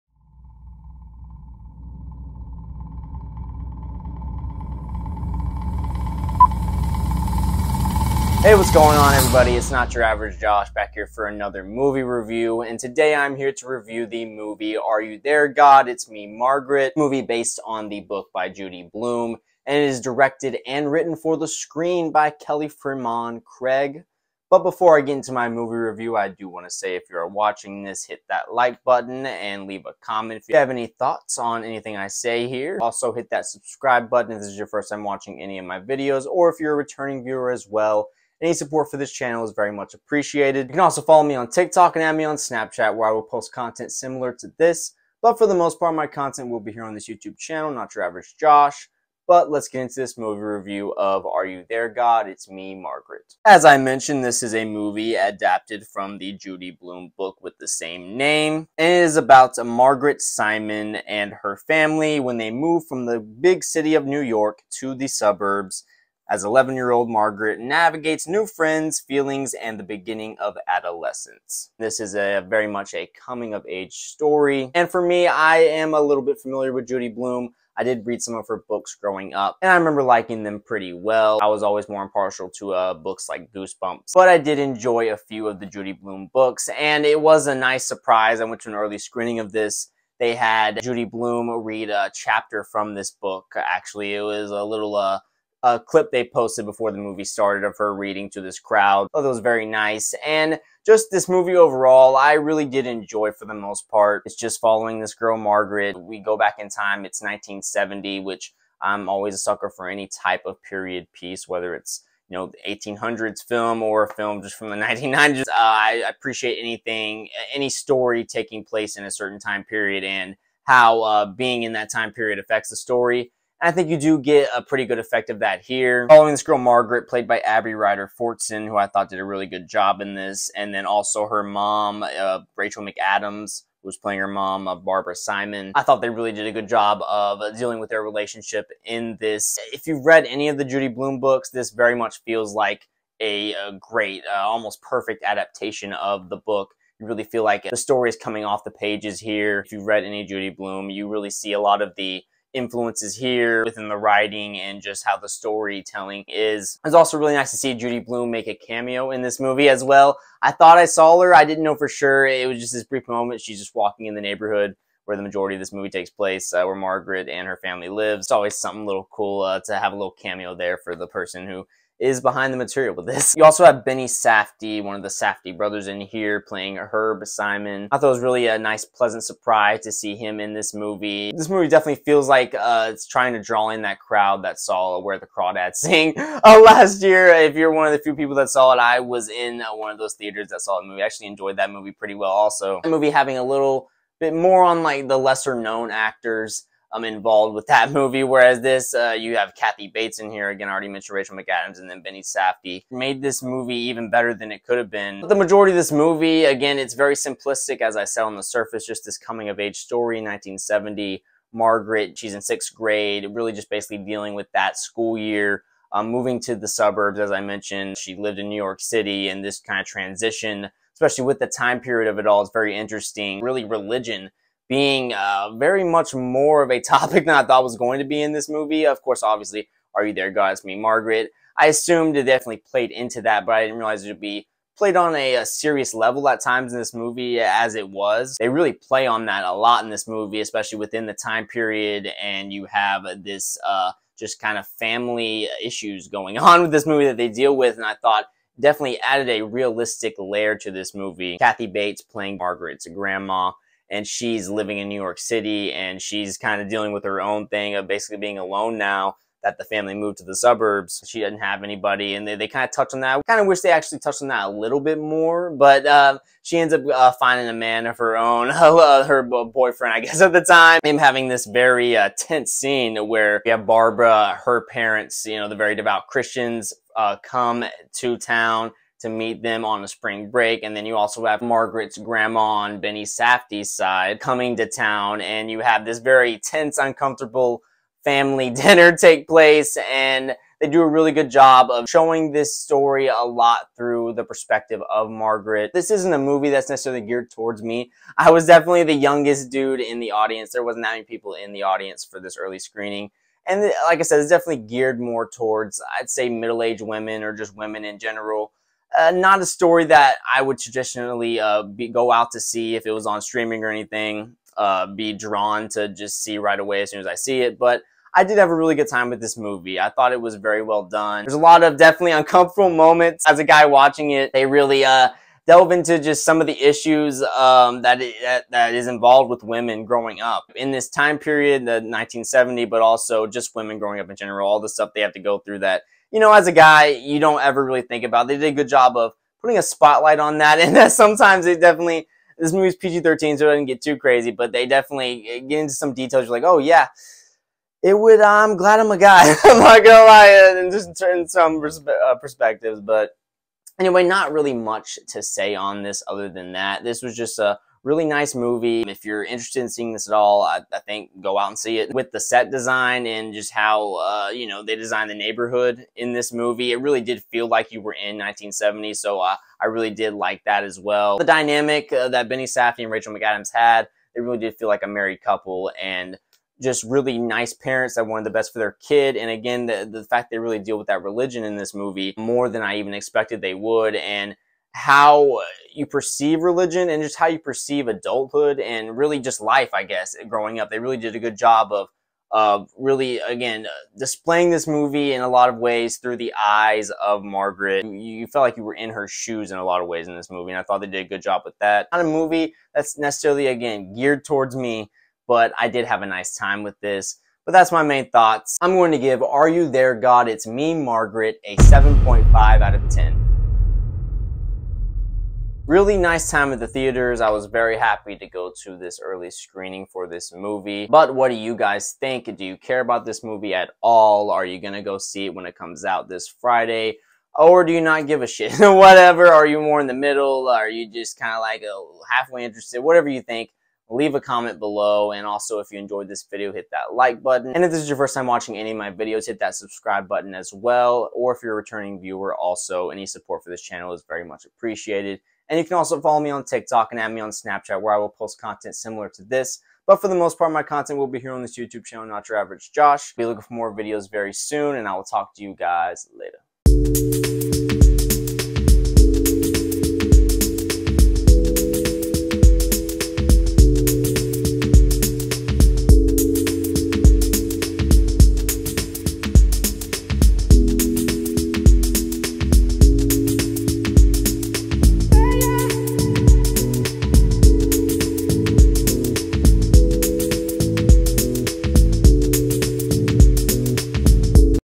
Are You There God? It’s Me Margaret. - MOVIE REVIEW
This is my review of Are You There God? It’s Me Margaret!! I appreciate anyone who is watching this! This is my hobby and passion!

TIME CODES:
00:00 Intro
00:50 Review
06:40 My Rating and Outro